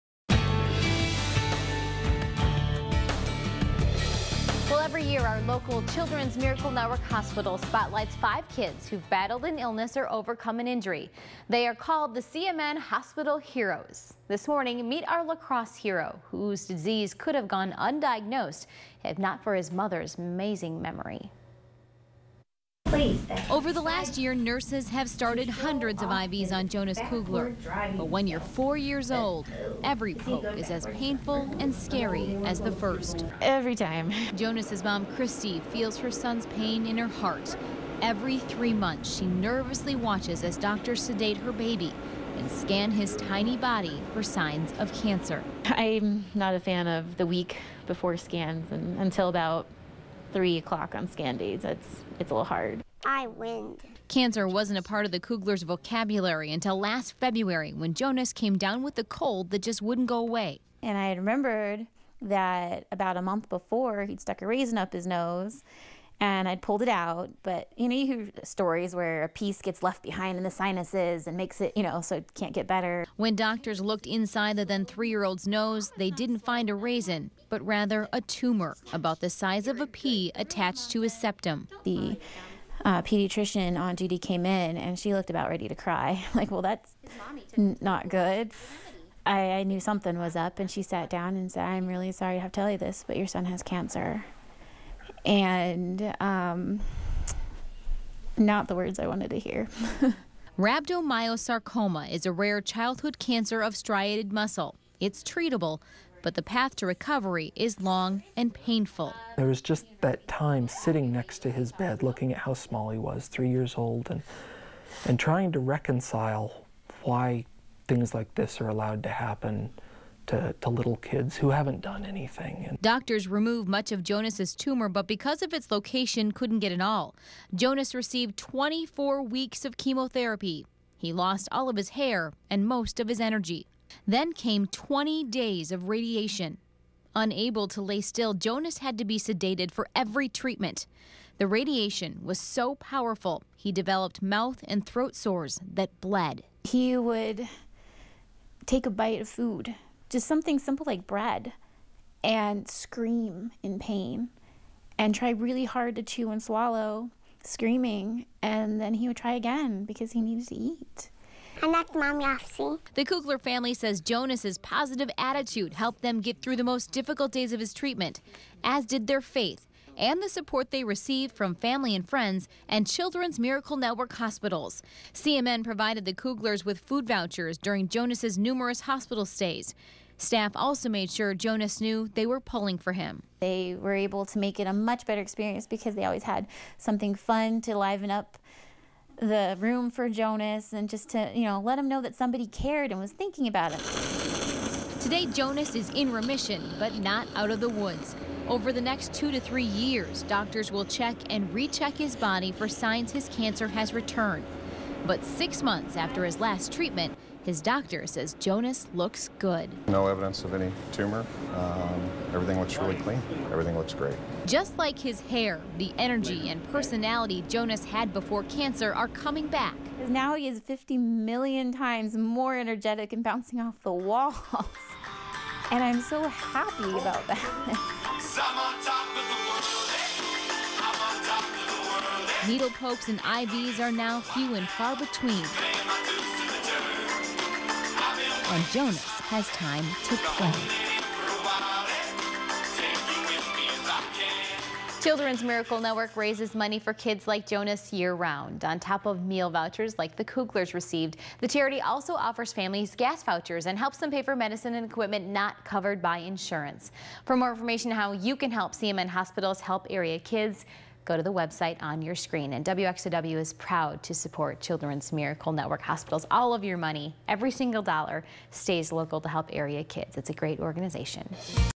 Children's Miracle Network Hospitals® hero Jonas Kugler's story
Ask Jonas Kugler of La Crosse, who is not yet 4 years old, and he'll tell you it is rhabdomyosarcoma. It's not a word you'd expect a kid to know, but then most kids aren't battling this beast—cancer. Jonas insisted on learning how to say the word. Maybe it's his way to know his enemy.